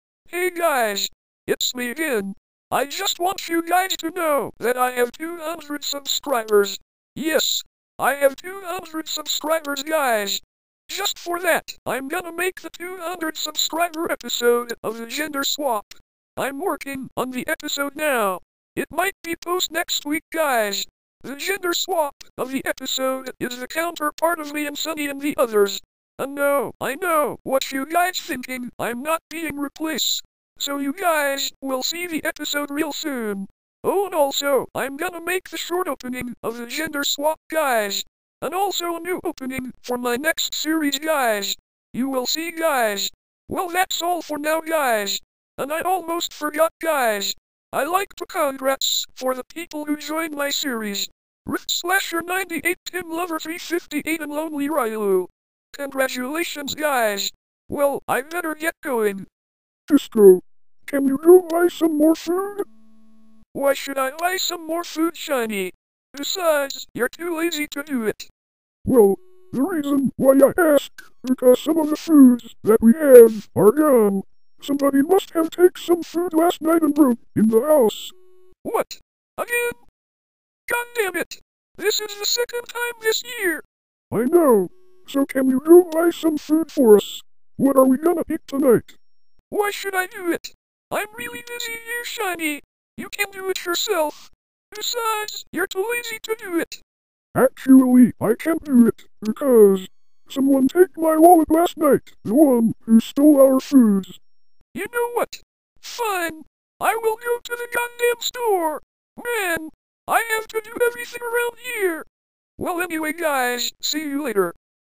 my 200 sub sprite news
congratulations bullet rift and sheng! im gonna get rid of the 5 characters soon guys because many people keep asking me that they want to join my series besides i dont wanna have too much characters because its alot of work to put them together in my series guys